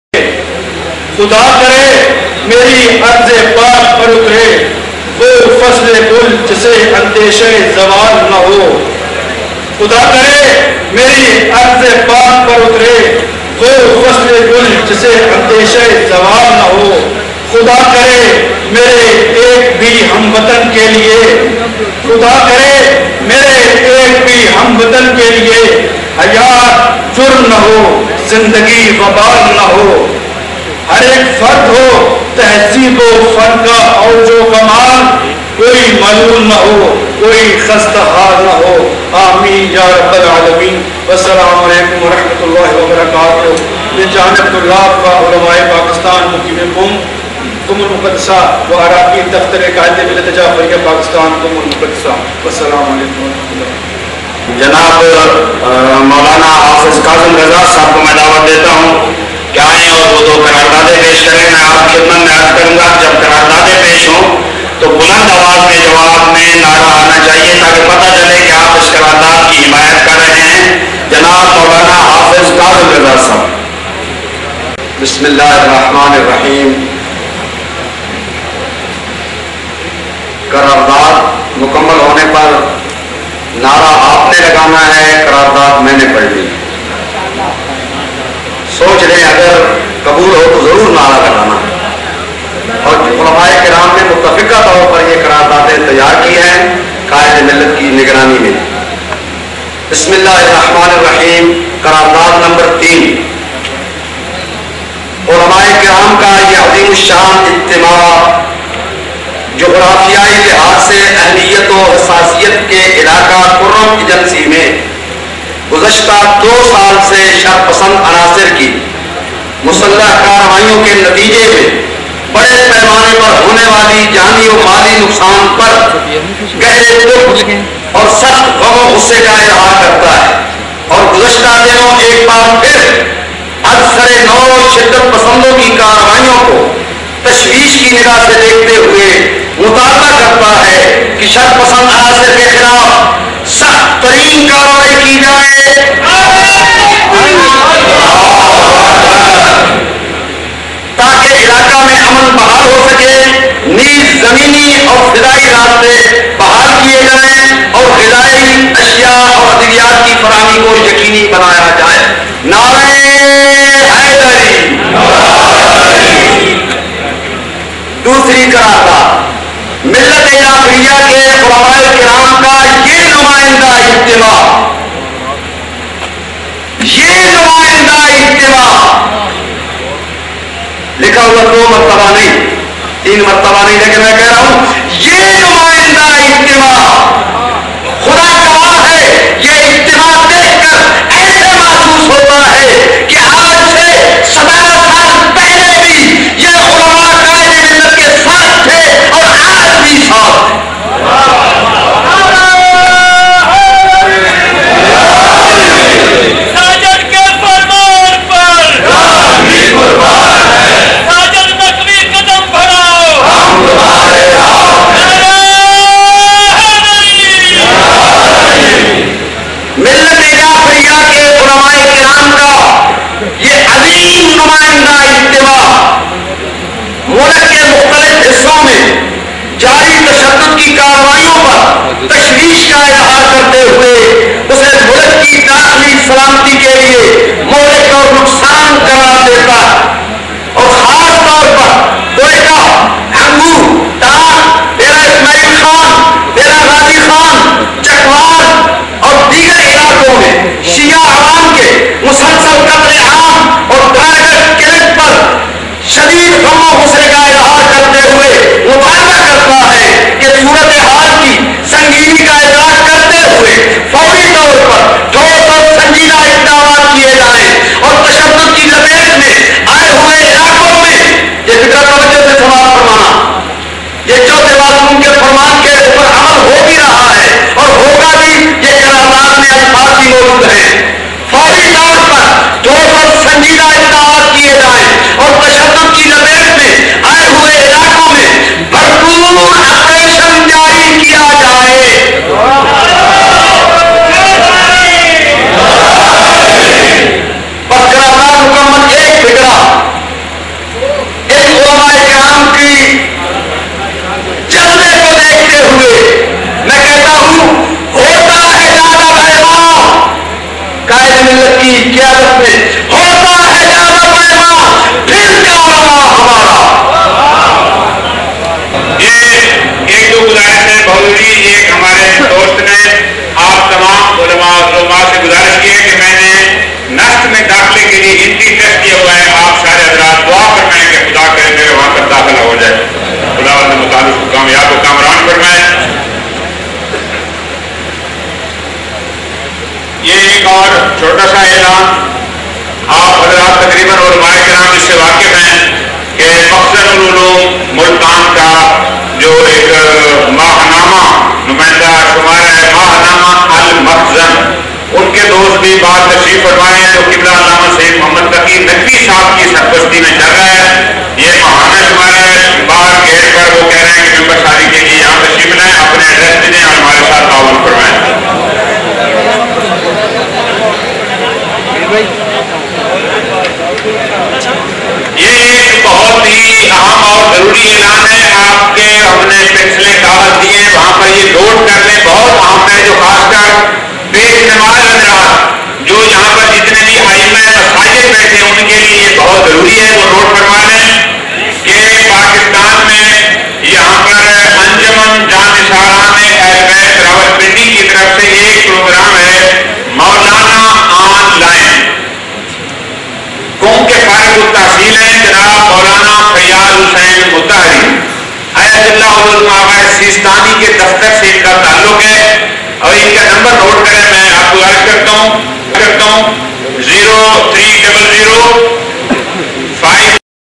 Part 25/38 Ulema Convention Islamabad 5 July, 2009 presided by Allama Syed Sajid Ali Naqvi
The Ulema Convention Held in Islamabad on 5 July, 2009 organized by Shia Ulema Council Pakistan. Almost all of the well known ulemas attended the one day convention presided by Quaid e Muhtarram Allama Syed Sajid Ali Naqvi. Some well known ulemas who made the speech are Sheikh Mohsin Najafi, Allama Abdul Jaleel Nqvi, Allama Syed Muhammad Taqi Shah Naqvi, Allama Abbas Kumaili, Allama Nazir Abbas, Allama Altaf Hussaini and  Allama Syed Sajid Ali Naqvi.